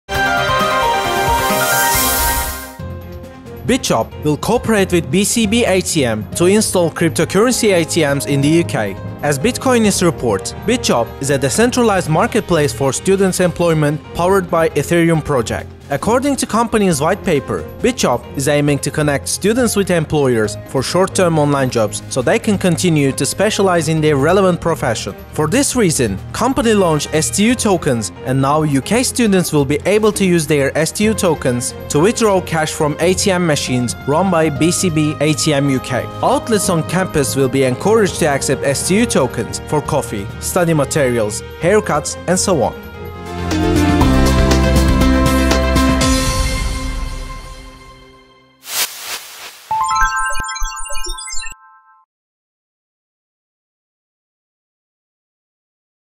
KCN BitJob will set crypto ATMs in the UK
BitJob will cooperate with BCB-ATM to install cryptocurrency ATMs in the UK. As Bitcoinist reports, Bitjob, is a decentralized marketplace for student employment, powered by Ethereum project. According to companies’ whitepaper, BitJob is aiming to connect students with employers for short-term online jobs so they can continue to specialize in their relevant profession. For this reason, company launched STU tokens and now UK students will be able to use their STU tokens to withdraw cash from ATM machines run by BCB-ATM UK. Outlets on campus will be encouraged to accept STU tokens for coffee, study materials, haircuts, and so on.